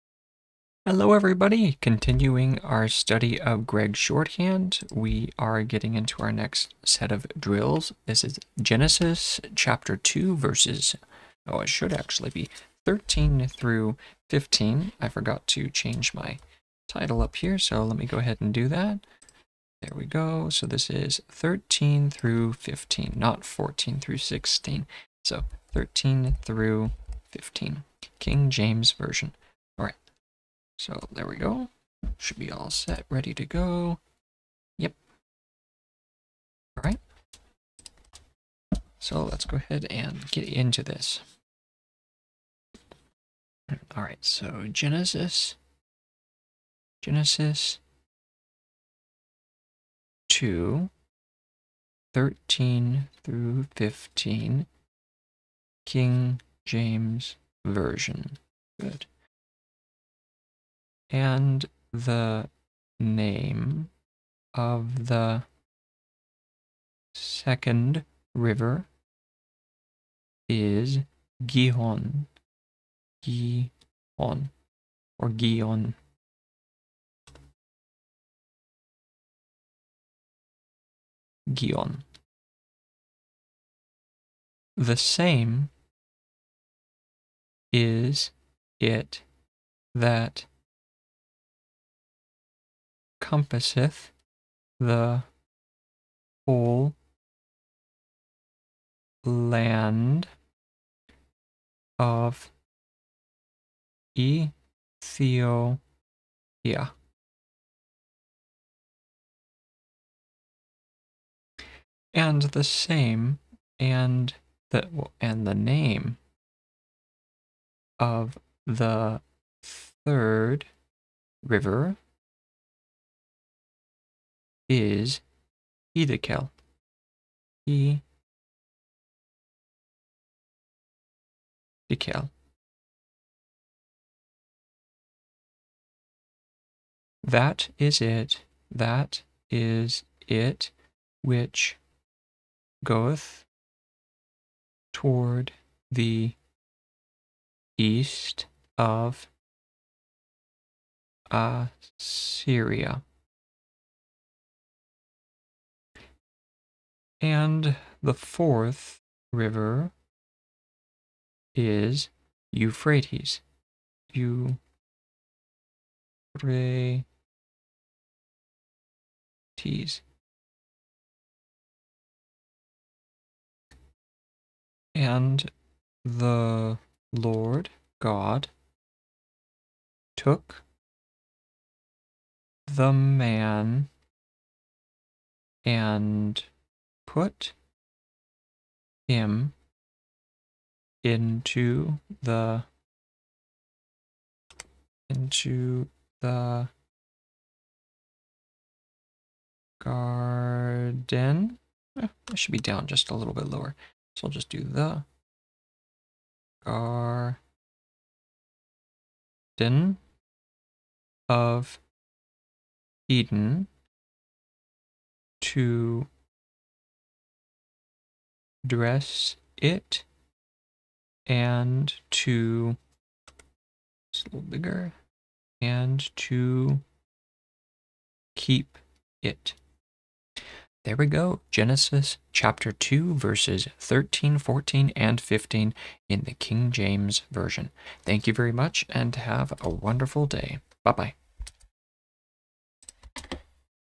The Bible in Gregg Shorthand - Genesis 2:13-15 (KJV)
A daily drill exercise to aid in the learning and study of Gregg Anniversary Shorthand.

Nathan

Video recording information:
Microphone:  Zoom ZDM-1